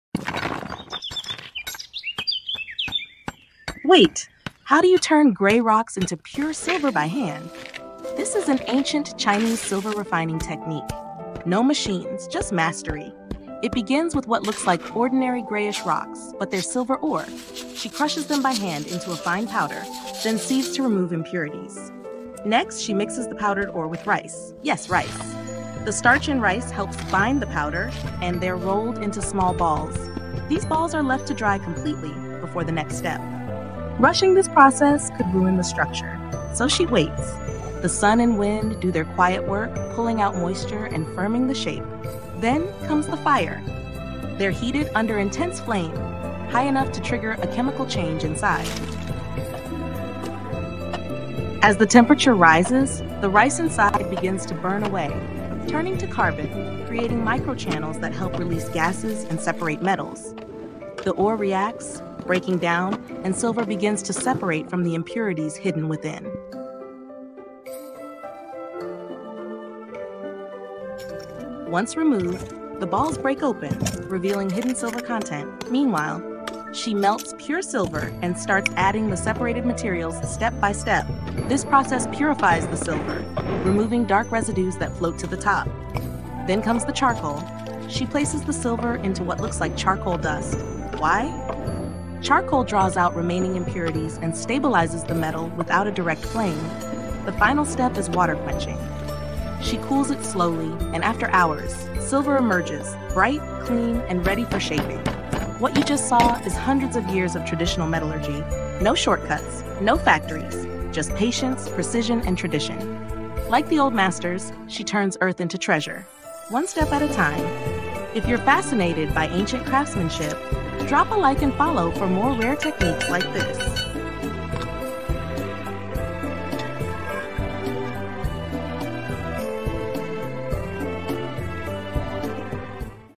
How Ancient Chinese Refined Silver by Hand (Step by Step)handmade
This video reveals an ancient Chinese silver refining method that has been passed down for centuries, using traditional techniques without any machines or chemicals. Starting from raw silver ore — crushed, sieved, and mixed with rice — each step unlocks the hidden metal through a slow, precise process rooted in ancient metallurgy.Watch how rice isn’t just a binder, but a key player in creating micro-channels that allow gases to escape and silver to separate from impurities. Every moment is deliberate — from hand-forming and drying the ore balls under the sun, to carefully controlling the flame, reading the color of heat, and purifying silver through charcoal and water quenching.This isn’t just a method — it’s a philosophy: slow craftsmanship, deep knowledge, and elemental transformation. For those fascinated by traditional metalwork, historical chemistry, and ancient Chinese heritage, this is a rare glimpse into a process few have seen in action. A huge thank you to @归来客 for sharing this incredible traditional craft and hardwork. Her dedication to preserving these ancient skill methods is truly inspiring.t’s a living tradition passed down through generations.